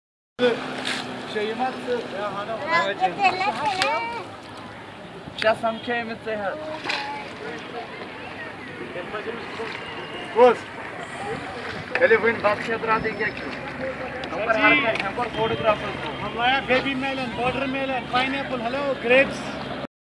Dal Lake Sri nagar, Kashmir
Dal Lake is a lake in Srinagar, the summer capital of Jammu and Kashmir. The urban lake, which is the second largest in the state, is integral to tourism and recreation in Kashmir and is named the "Jewel in the crown of kashmir"